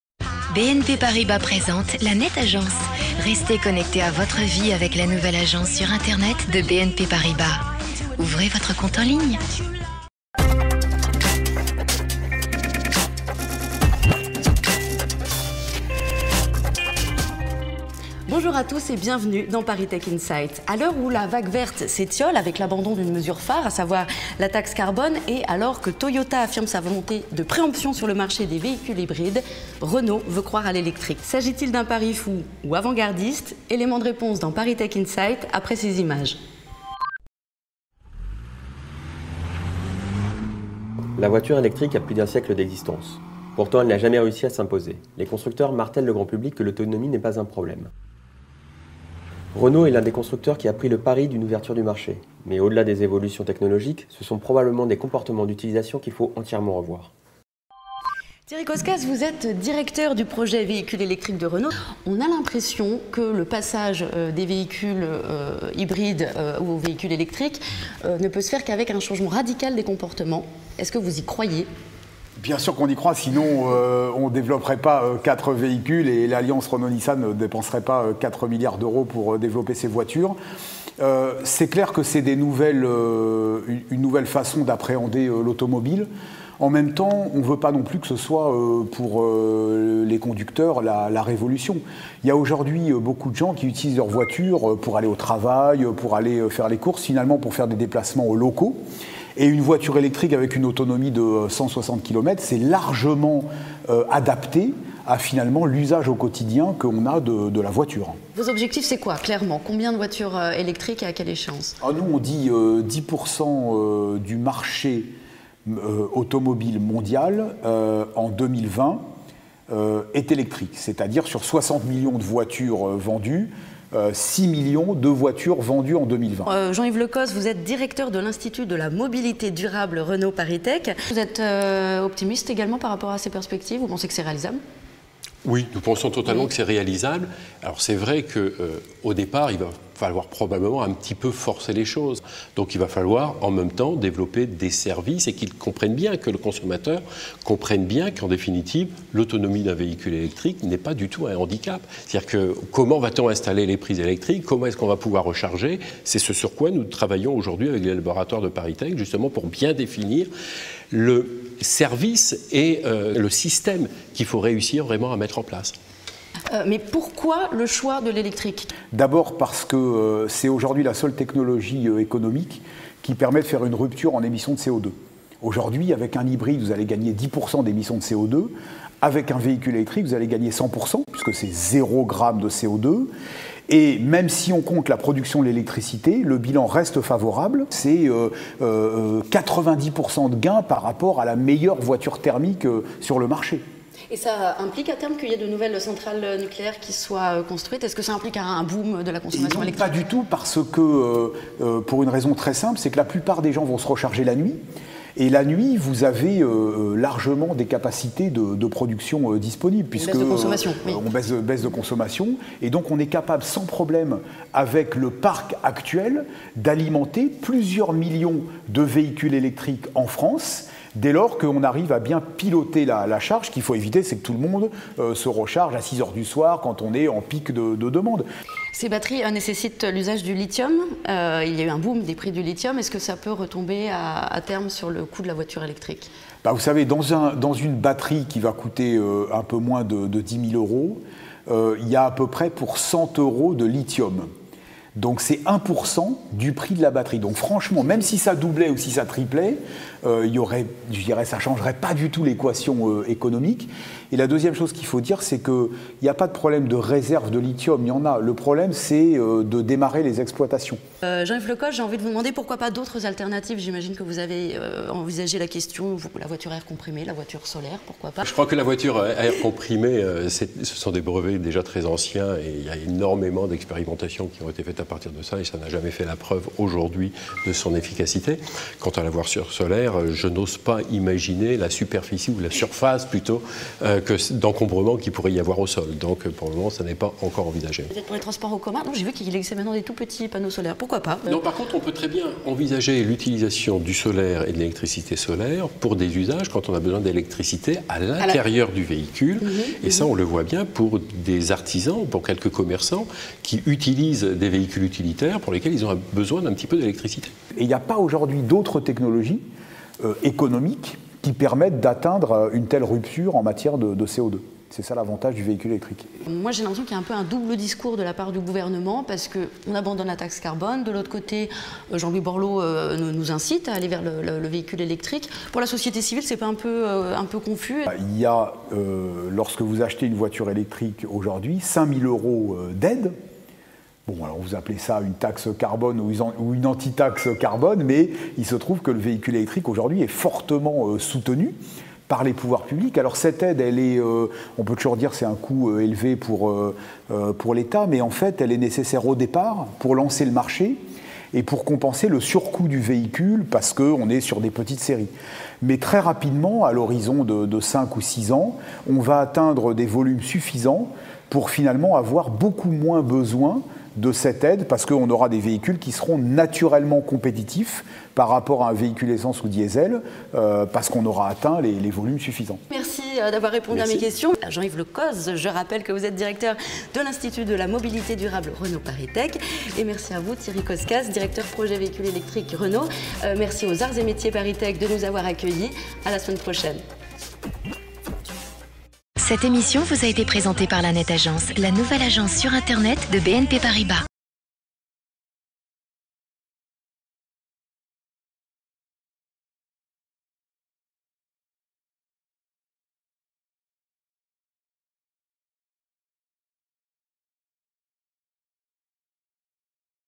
Véhicules électriques: Renault changera-t-il les mentalités?
Entre l'essoufflement de la vague verte, symbolisé par l'abandon de la taxe carbone, et la concurrence des véhicules hybrides, Renault parviendra-t-il à imposer sa vision du marché des voitures électriques et vaincre les réticences des consommateurs ?